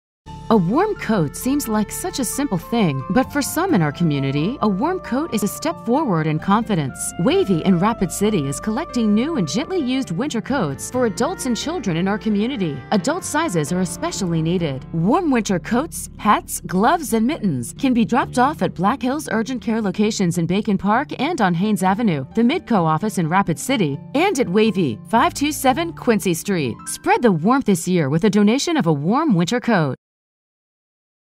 WAVI Warm Winter Coat
Donations are now being accepted for warm winter coats, hats, mittens and gloves for WAVI in Rapid City, Donations can be dropped off at Black Hills Urgent Care locations, Midco, and WAVI, 527 Quincy St. in Rapid City, SD. Spread the warmth this year by donating a women's winter coat!